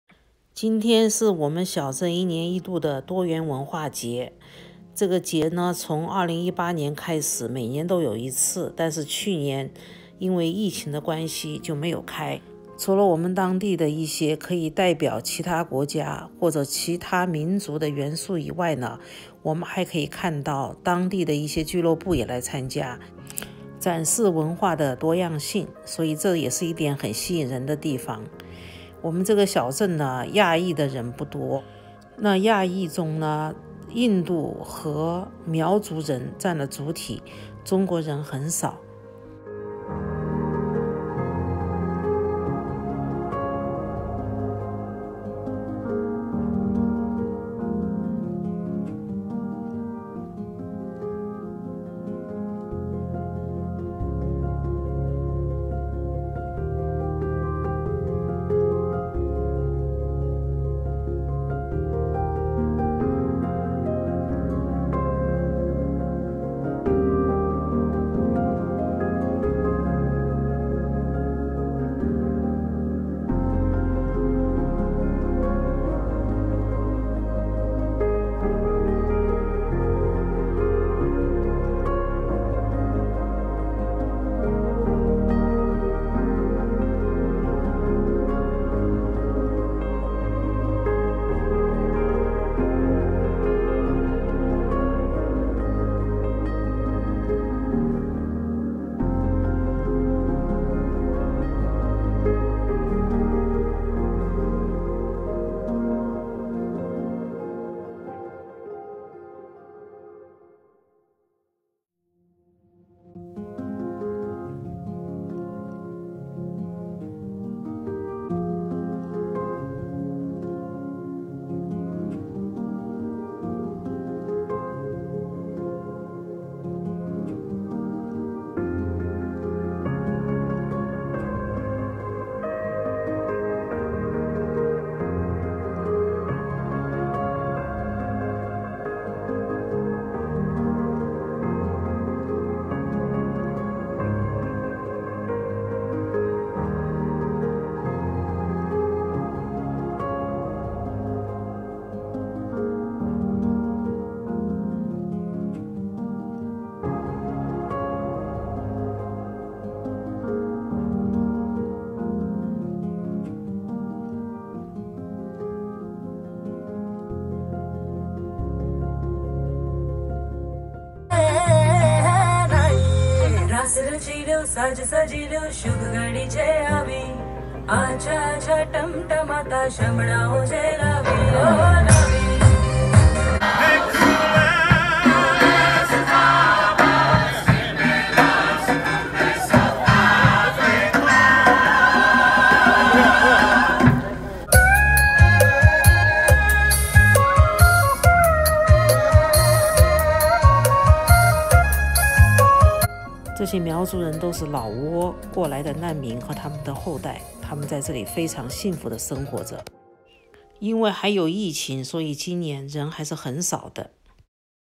看看小镇的多元文化 今年的多元文化节还在疫情中 人流稀少
参加展示的团队来自全县及附近的城市。由于疫情，没看到中国的节目和小吃，有点遗憾。